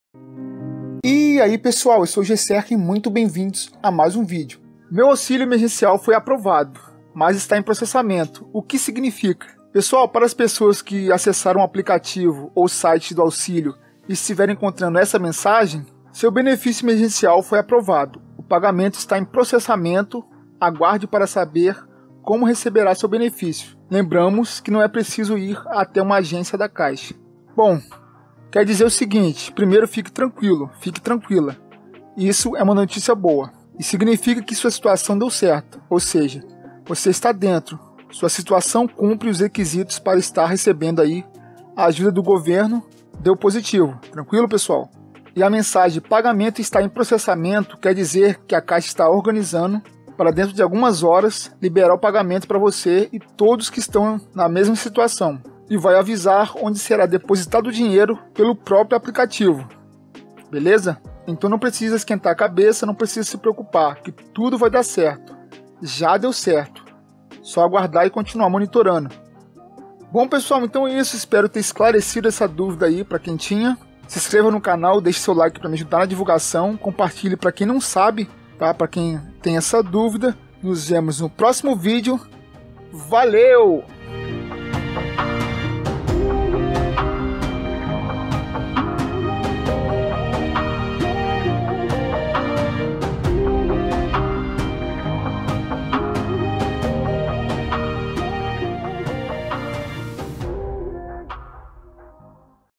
MEU AUXÍLIO EMERGENCIAL FOI APROVADO, MAS O PAGAMENTO ESTÁ EM PROCESSAMENTO...
Olá, bem-vindo(a) ao Canal Jessé Tutors!

Meu auxílio emergencial foi aprovado, mas está em processamento, o que isso significa?

Pessoal, para as pessoas que ao acessarem o app ou site do auxilio e estiverem encontrando essa mensagem: "Seu beneficio emergencial foi aprovado. O pagamento está em processamento, aguarde para saber como receberá seu benefício. Lembramos que não é necessário ir até uma agência da Caixa."

Bom, quer dizer o seguinte, primeiro fique tranquilo, fique tranquila, isso é uma notícia boa, e significa que sua solicitação deu certo, ou seja, você está dentro, sua situação cumpre os requisitos para estar recebendo a ajuda do governo, recebeu um positivo.

E a mensagem: (...)pagamento está em processamento(...), quer dizer que a Caixa está se organizando para dentro de algumas horas, liberar o pagamento pra você e todos que estão na mesma situação, e vai avisar onde será depositado o dinheiro pelo próprio aplicativo. Então, não precisa esquentar a cabeça, não precisa se preocupar, que tudo vai dá certo, já deu certo, só aguardar e continuar monitorando, certo?!

▶︎ Aprenda a fazer PROJETOS INCRÍVEIS DE MADEIRA rápido, fácil e descomplicado em pouco dias: